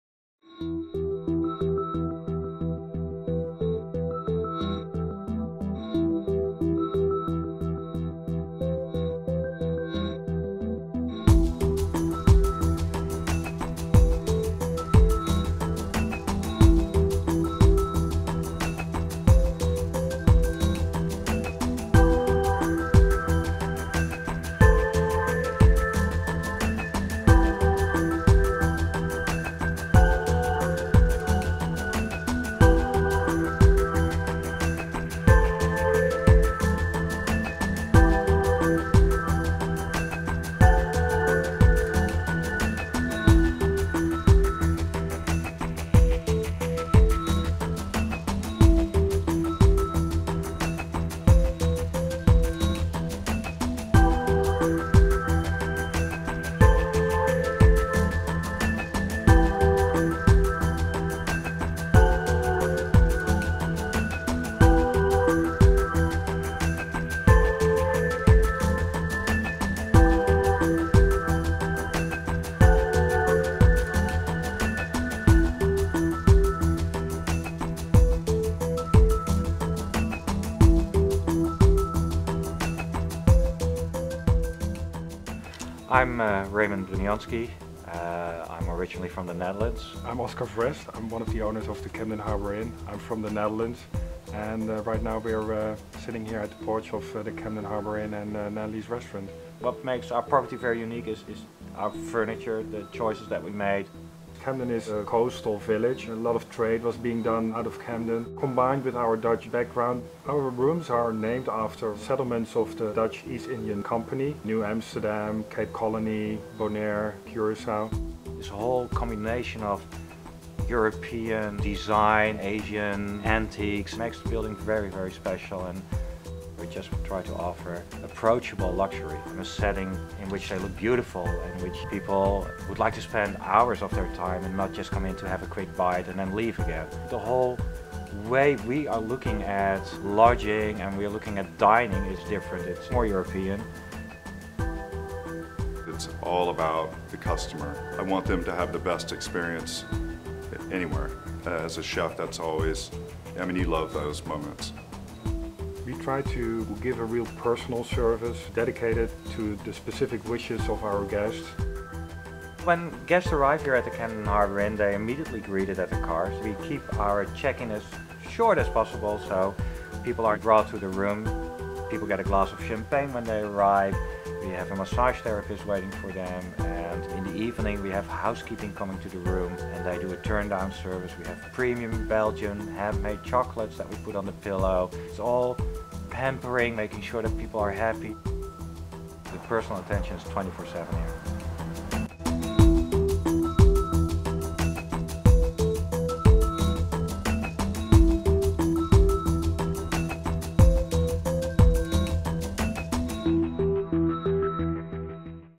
camden harbour inn
Lifestyle Lodging in Maine

Come to the luxurious Camden Harbour Inn and live a dream . . . indulge your sensibilities . . . let your imagination soar. An intimate boutique lifestyle hotel, the Inn is a sought-out oasis for those who wish to savor the finer aspects of travel, relaxation, discovery, and renewal.

The Inn is perched on a hilltop with 180-degree views of the village, harbor, mountains, Penobscot Bay, and evergreen islands. Take time to luxuriate in your bedchamber overlooking the water, dine on exquisite cuisine in Natalies award-winning restaurant, and experience the mind-body benefits of Zero Balancing in the spa.

It is less than a five-minute walk to the heart of Camden, where you will find a delightful collection of unusual shops, galleries, and restaurants, as well as traditional windjammers and power vessels waiting to take you sightseeing on the Bay.

All 18 rooms and suites are fitted with luxurious amenities including king size featherbeds, flat screen TV's with DVD, CD player, coffee maker, iron and ironing board, private baths with tubs and showers, fluffy bathrobes, complimentary wireless internet, IPOD docking station, fully stocked mini bars and more. All rooms have breathtaking views over the harbor and mount Battie and/or Penobscot Bay. Some rooms have private decks and/or fire places